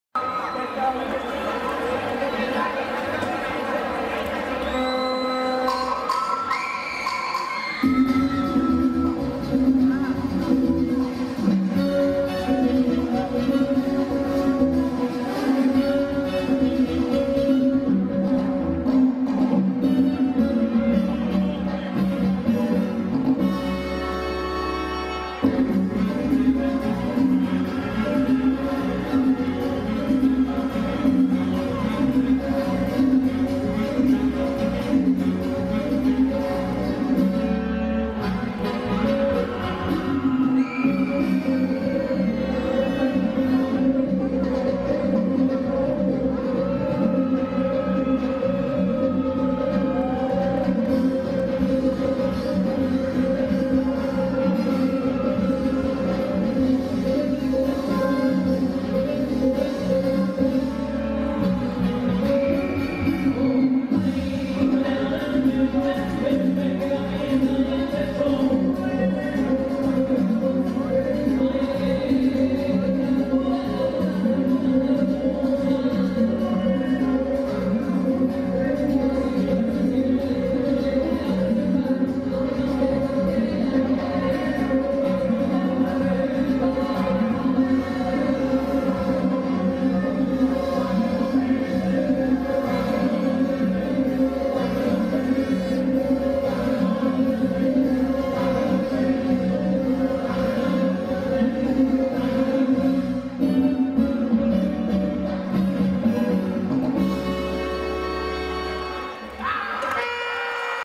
Katherine Rosales y Jonathan Quiñonez
Semifinales Parejas On1 Amateur, XI Festival Mundial de Baile Salsa de Cali 2016
Pasos Básicos de Salsa Caleña, Bailar Salsa de Cali Colombia